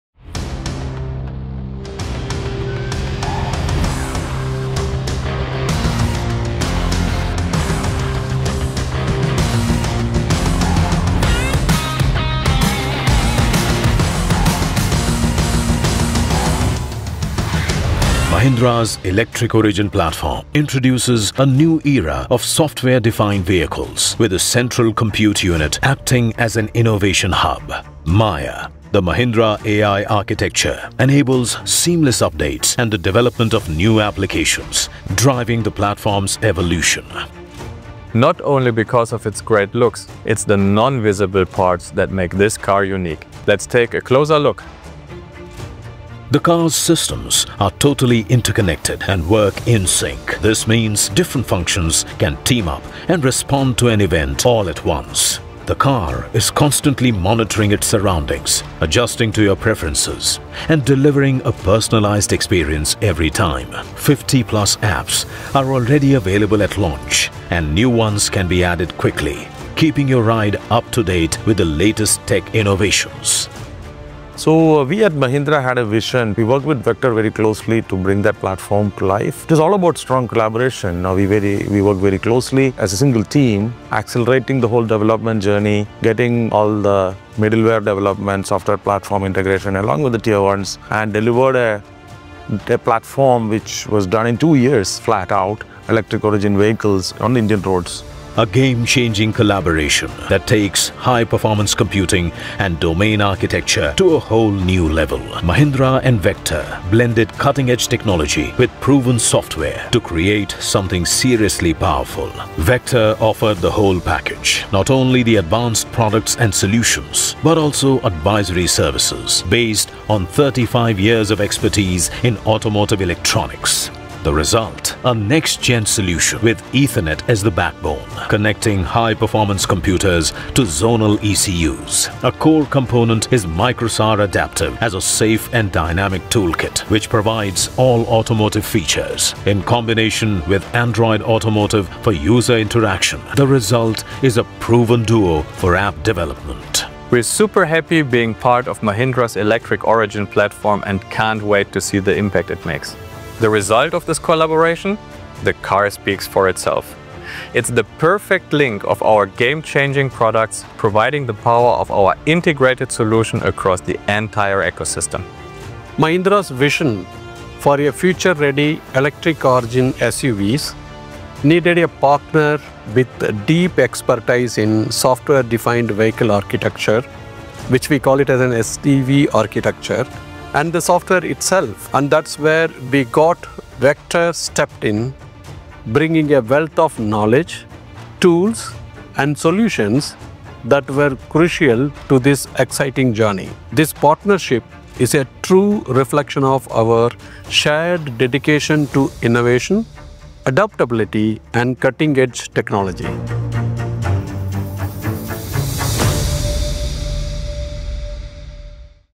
Mahindra & Vector Collaboration for SDV: Driving Innovation Together
Mahindra and Vector have created a groundbreaking platform for Software-Defined Vehicles (SDVs) in just two years. With over 50 apps available at launch, a fully interconnected vehicle architecture and the integration of MICROSAR Adaptive in combination with Android Automotive, the Electric Origin Platform sets a new benchmark in automotive innovation.

This success story showcases how strong partnerships, deep technological expertise and a shared spirit of innovation are shaping the future of mobility.

More information about our SDV solution and the possibility to get in contact, you can find on our